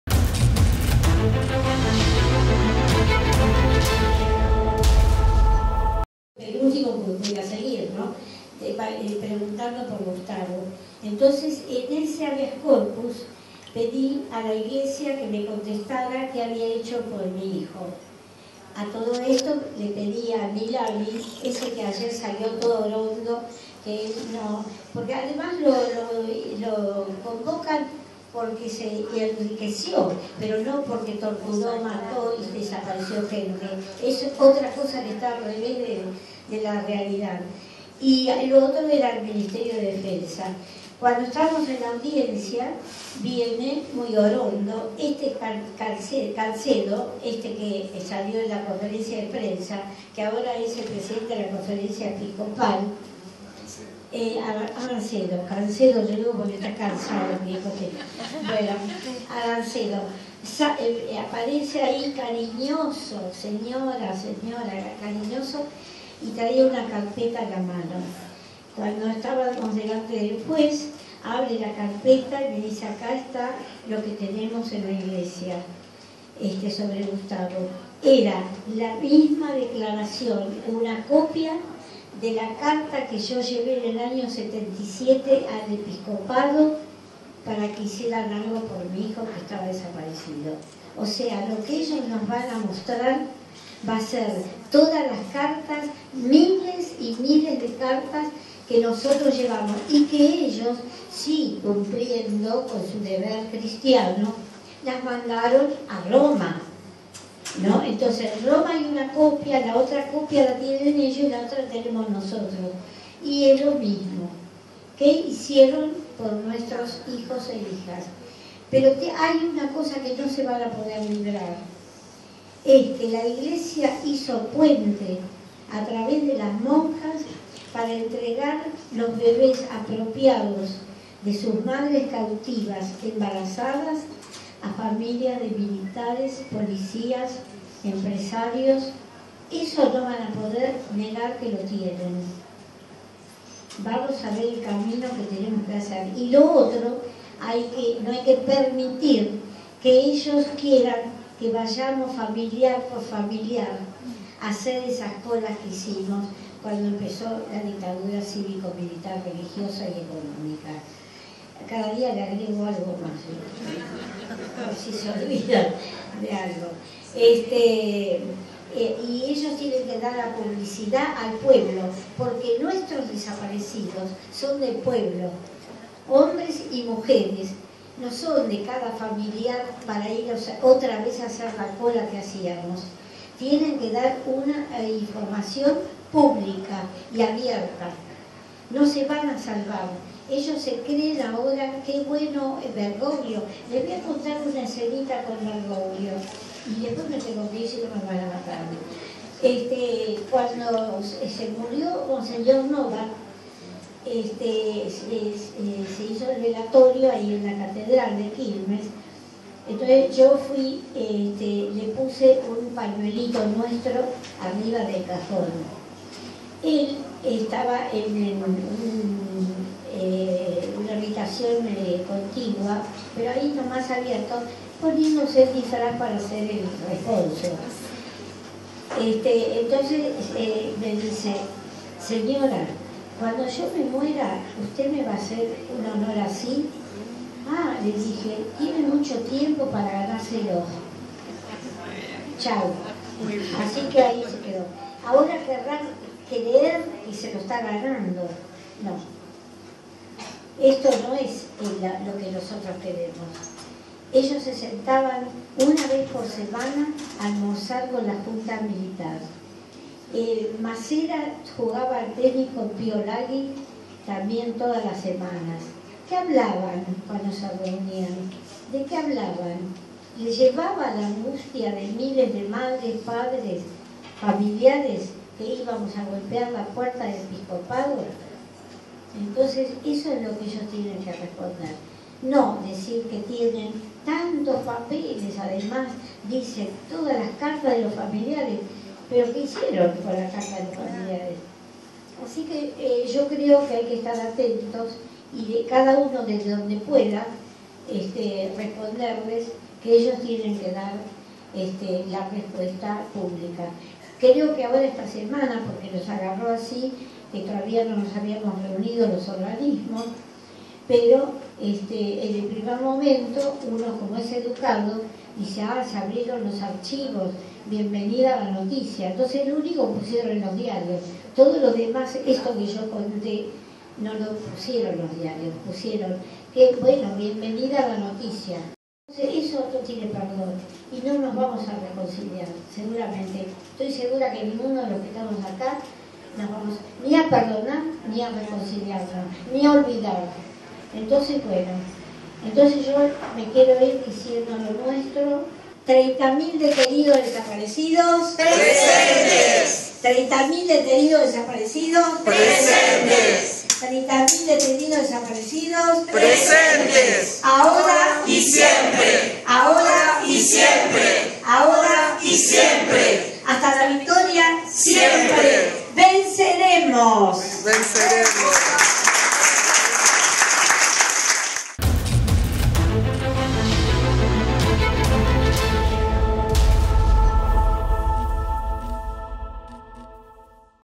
Nora Cortiñas desenmascara la apertura de archivos de la Iglesia
En el marco de las Cátedras Bolivarianas de la taberna internacionalista vasca de Buenos Aires, Nora Cortiñas, Madres de Plaza de Mayo Linea Fundadora, desmonta noticia sobre la apertura de los archivos de la dictadura por parte de la iglesia católica.